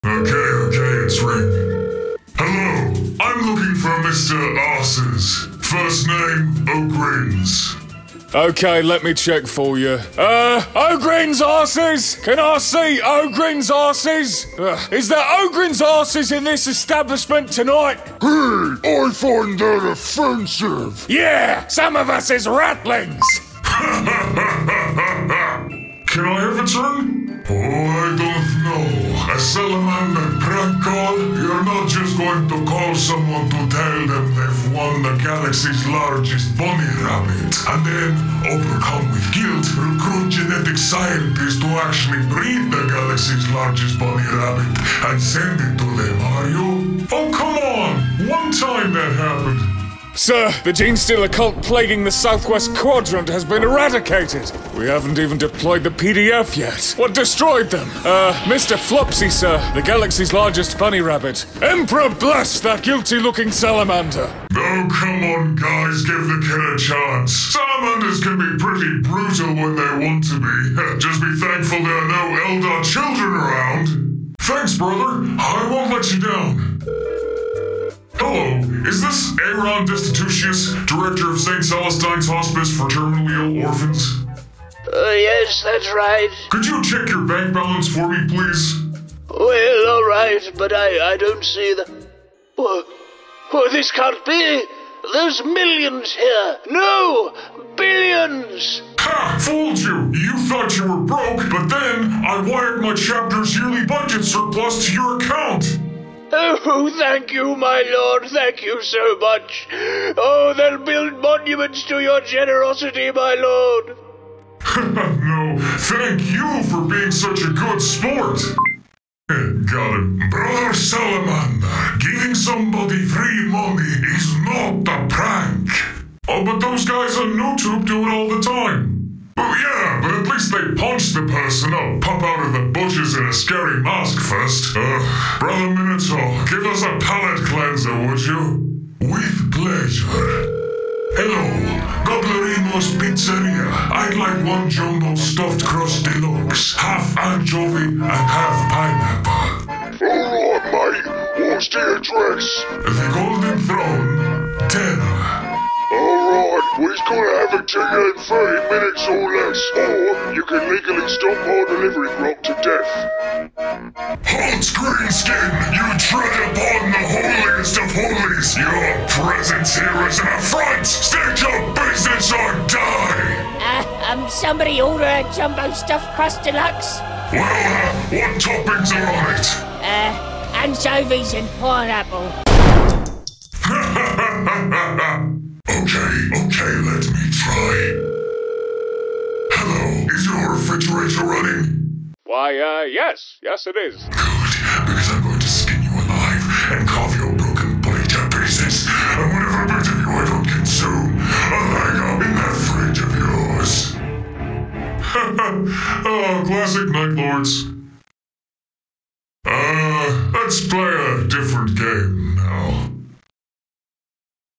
Space Marine Prank Calls | a Warhammer 40k story
In the Grim Darkness of the far future, there sometimes isn't an awful lot going on. So to pass the time, our intrepid Astartes of the Marines Malevolent, Minotaurs and Salamanders (and also the Night Lords are there too for some reason) have banded together to fight the greatest enemy of the Imperium of Man: boredom! But perhaps they're in for TOO much excitement...

Also face reveal if you want to dox me

Litany Algorithmicus:
space marines, minotaurs, marines malevolent, salamanders, night lords, prank call, prank call memes, 40k memes, warhammer 40k memes, ogryn, darktide, darktide ogryns